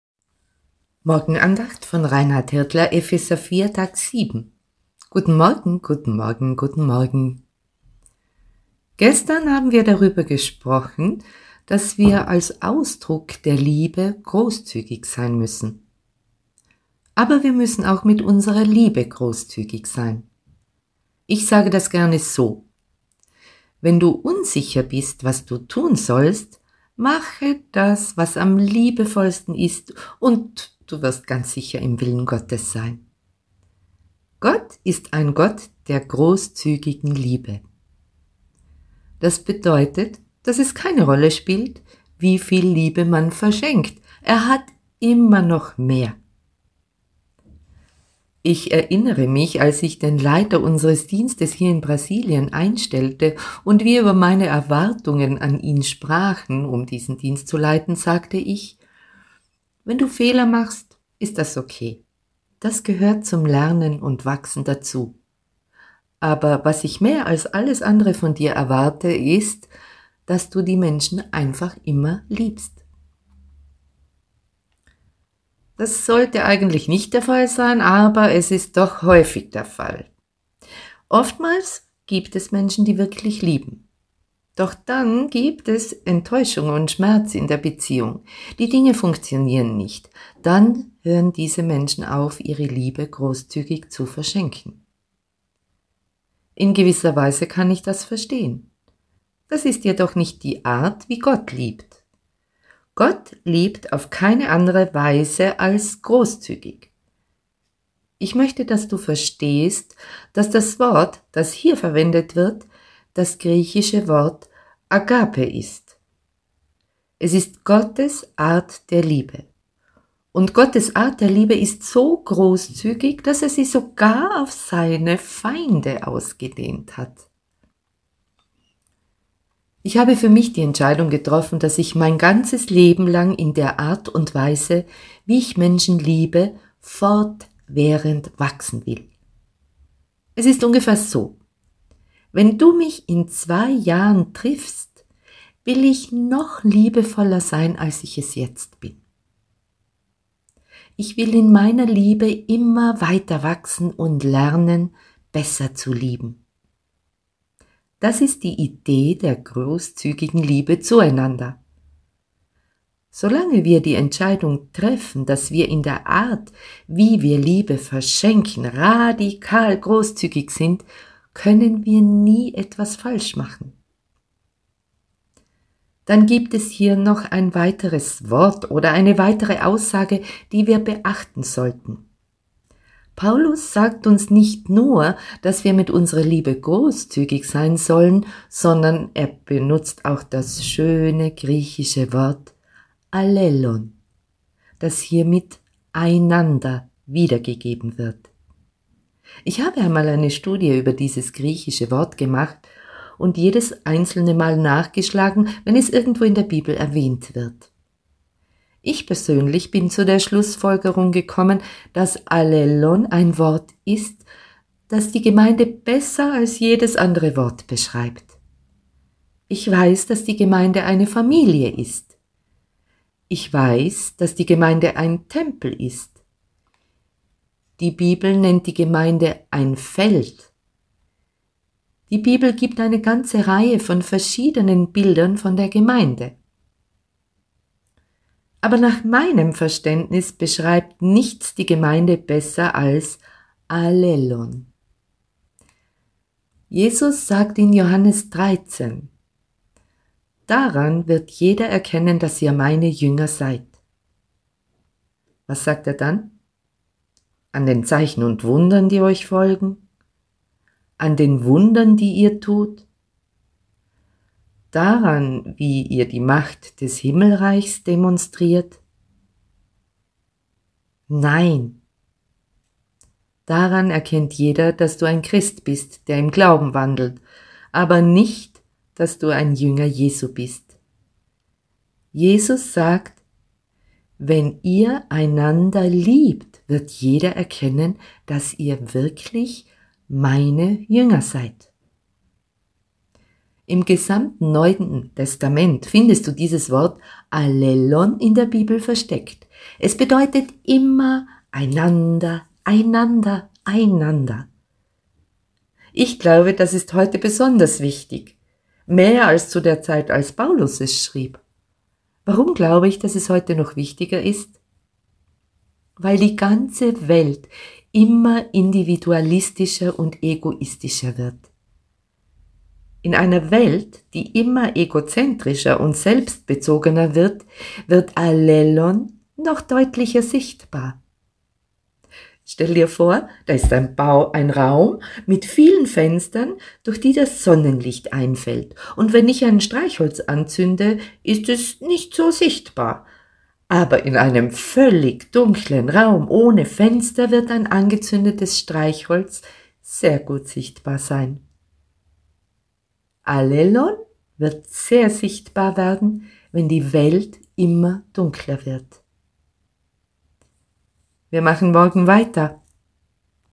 Tag 7 💛 Epheser 4,2 gelesen
Guten Morgen, guten Morgen, guten Morgen!
Gestern haben wir darüber gesprochen, dass wir als Ausdruck der Liebe großzügig sein müssen. Aber wir müssen auch mit unserer Liebe großzügig sein. Ich sage das gerne so: Wenn du unsicher bist, was du tun sollst, mache das, was am liebevollsten ist und du wirst ganz sicher im Willen Gottes sein. Gott ist ein Gott der großzügigen Liebe. Das bedeutet, dass es keine Rolle spielt, wie viel Liebe man verschenkt, Er hat immer noch mehr.
Ich erinnere mich, als ich den Leiter unseres Dienstes hier in Brasilien einstellte und wir über meine Erwartungen an ihn sprachen, um diesen Dienst zu leiten, sagte ich: „Wenn du Fehler machst, ist das okay. Das gehört zum Lernen und Wachsen dazu. Aber was ich mehr als alles andere von dir erwarte, ist, dass du die Menschen einfach immer liebst.“
Das sollte eigentlich nicht der Fall sein, aber es ist doch häufig der Fall: Oftmals gibt es Menschen, die wirklich lieben. Doch dann gibt es Enttäuschung und Schmerz in der Beziehung; die Dinge funktionieren nicht, dann hören diese Menschen auf, ihre Liebe großzügig zu verschenken. In gewisser Weise kann ich das verstehen. Das ist jedoch nicht die Art, wie Gott liebt. Gott liebt auf keine andere Weise als großzügig. Ich möchte, dass du verstehst, dass das Wort, das hier verwendet wird, das griechische Wort ‚agapé‘ ist. Es ist Gottes Art der Liebe. Und Gottes Art der Liebe ist so großzügig, dass Er sie sogar auf Seine Feinde ausgedehnt hat.
Ich habe für mich die Entscheidung getroffen, dass ich mein ganzes Leben lang in der Art und Weise, wie ich Menschen liebe, fortwährend wachsen will. Es ist ungefähr so: Wenn du mich in zwei Jahren triffst, will ich noch liebevoller sein als ich es jetzt bin. Ich will in meiner Liebe immer weiterwachsen und lernen, besser zu lieben. Das ist die Idee der großzügigen Liebe zueinander. Solange wir die Entscheidung treffen, dass wir in der Art, wie wir Liebe verschenken, radikal großzügig sind, können wir nie etwas falsch machen.
Dann gibt es hier noch ein weiteres Wort oder eine weitere Aussage, die wir beachten sollten. Paulus sagt uns nicht nur, dass wir mit unserer Liebe großzügig sein sollen, sondern er benutzt auch das schöne griechische Wort ‚allélón‘, das hier mit ‚einander‘ wiedergegeben wird. Ich habe einmal eine Studie über dieses griechische Wort gemacht und jedes einzelne Mal nachgeschlagen, wenn es irgendwo in der Bibel erwähnt wird. Ich persönlich bin zu der Schlussfolgerung gekommen, dass ‚allélón‘ ein Wort ist, das die Gemeinde besser als jedes andere Wort beschreibt. Ich weiß, dass die Gemeinde eine Familie ist, ich weiß, dass die Gemeinde ein Tempel ist. Die Bibel nennt die Gemeinde ein Feld. Die Bibel gibt eine ganze Reihe von verschiedenen Bildern von der Gemeinde. Aber nach meinem Verständnis beschreibt nichts die Gemeinde besser als ‚allélón‘.
Jesus sagt in Johannes 13: „Daran wird jeder erkennen, dass ihr meine Jünger seid.“ Was sagt Er dann? An den Zeichen und Wundern, die euch folgen? An den Wundern, die ihr tut? Daran, wie ihr die Macht des Himmelreichs demonstriert? Nein. Daran erkennt jeder, dass du ein Christ bist, der im Glauben wandelt, aber nicht, dass du ein Jünger Jesu bist. Jesus sagt: „Wenn ihr einander liebt, wird jeder erkennen, dass ihr wirklich Meine Jünger seid.“
Im gesamten Neuen Testament findest du dieses Wort ‚allélón‘ in der Bibel versteckt. Es bedeutet immer: Einander, einander, einander. Ich glaube, das ist heute besonders wichtig – mehr als zu der Zeit, als Paulus es schrieb. Warum glaube ich, dass es heute noch wichtiger ist? Weil die ganze Welt immer individualistischer und egoistischer wird. In einer Welt, die immer egozentrischer und selbstbezogener wird, wird ‚allélón‘ noch deutlicher sichtbar.
Stell dir vor, da ist ein Raum mit vielen Fenstern, durch die das Sonnenlicht einfällt, und wenn ich ein Streichholz anzünde, ist es nicht so sichtbar. Aber in einem völlig dunklen Raum ohne Fenster wird ein angezündetes Streichholz sehr gut sichtbar sein. ‚Allélón‘ wird sehr sichtbar werden, wenn die Welt immer dunkler wird.
Wir machen morgen weiter.

Epheser 4,2
In sanfter Demut und stiller Geduld zeigt einander stets Sanftmut und großzügige Liebe. Besonders denen gegenüber, die eure Geduld auf die Probe stellen.
Möchtest du mehr über uns erfahren?